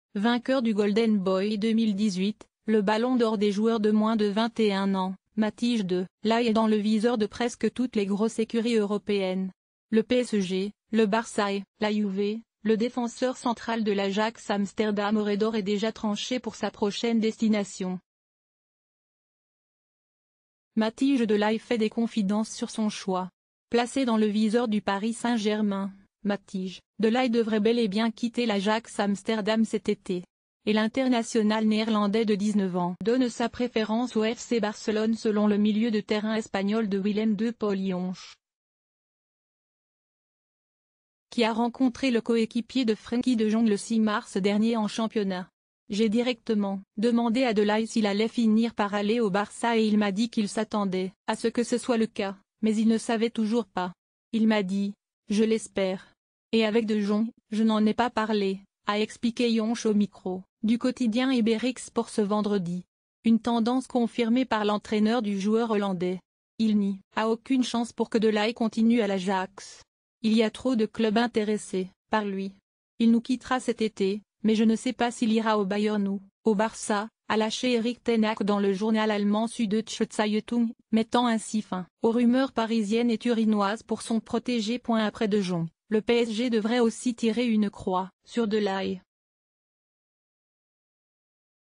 Ajax : PSG, Barça, Juve, le choix de Matthijs de Ligt est connu !
Foot-mercato-day - La dernière chaîne pour mettre à jour les nouvelles du football du jour